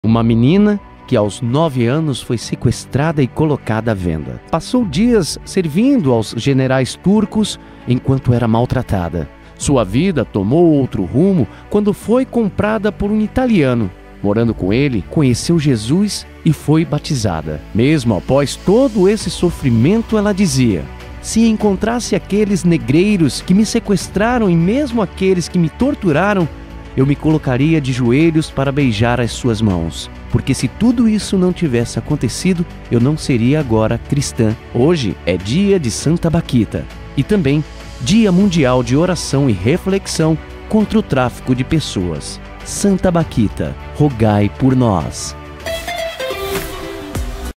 Dia Mundial da Oração e Reflexão Contra o Tráfico Humano
Hoje é dia de Santa Bakhita, e também, dia mundial da oração e reflexão contra o tráfico humano.